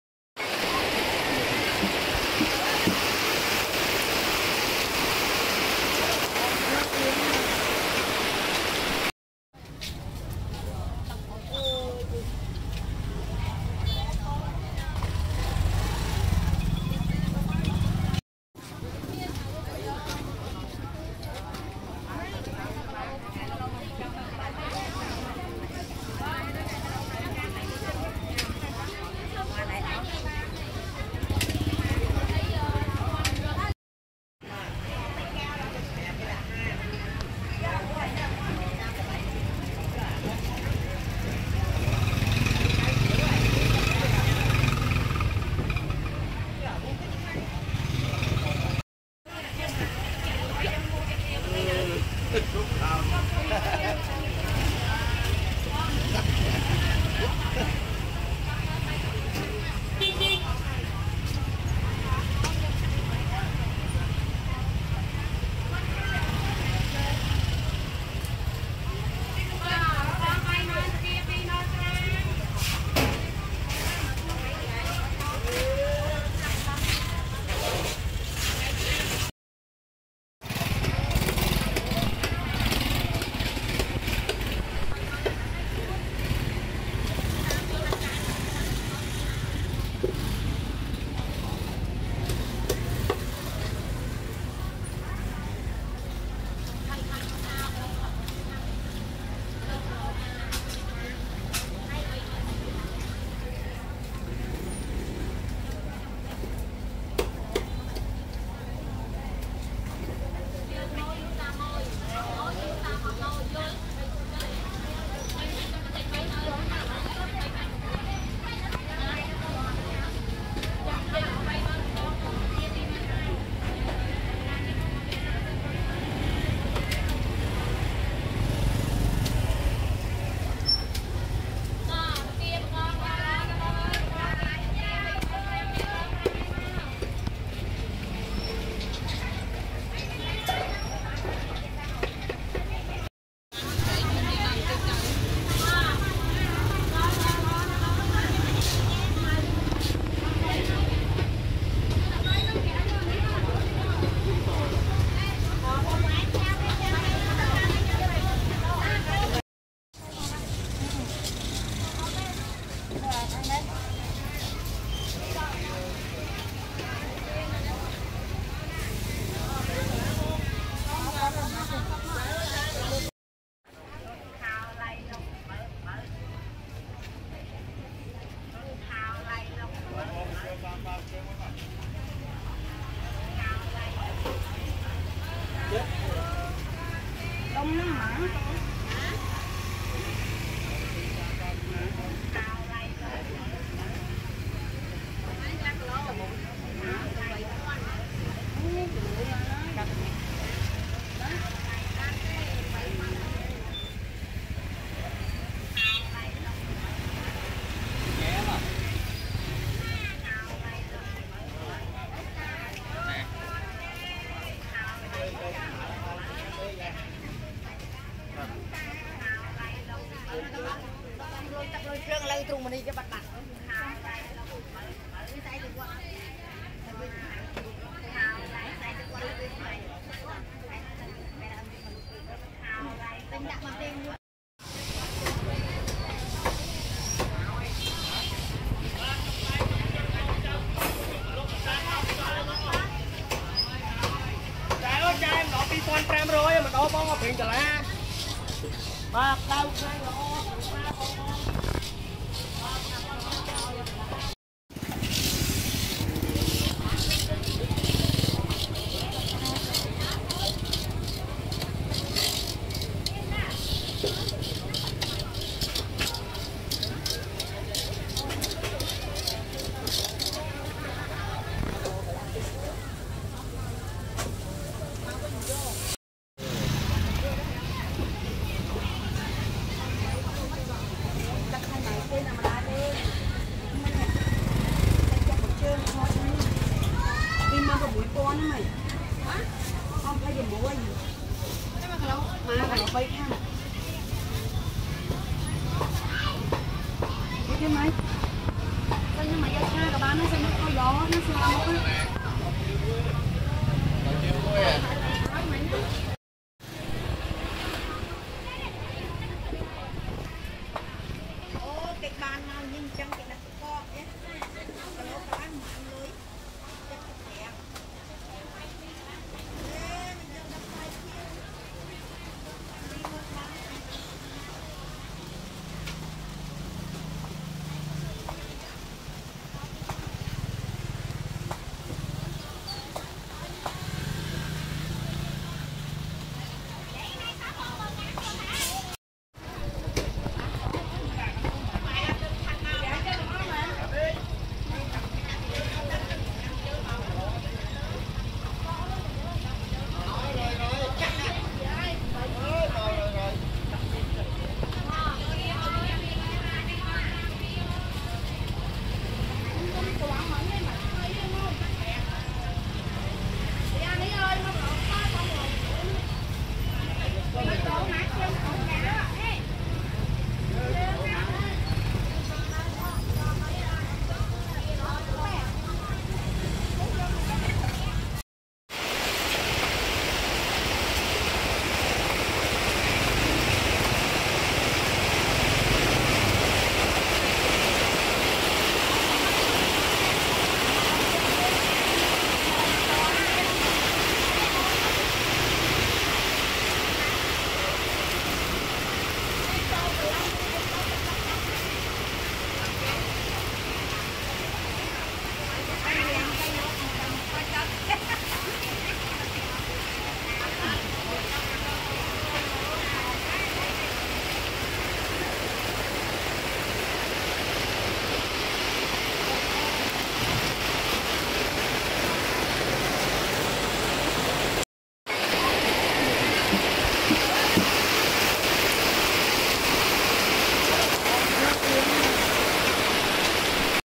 Traditional Market under raining in the morning
This is the view of Traditional Market in Phnom Penh, Cambodia under raining in the morning. Before raining, this market have a lots of food for sell like meat, vegetables and fresh fishes.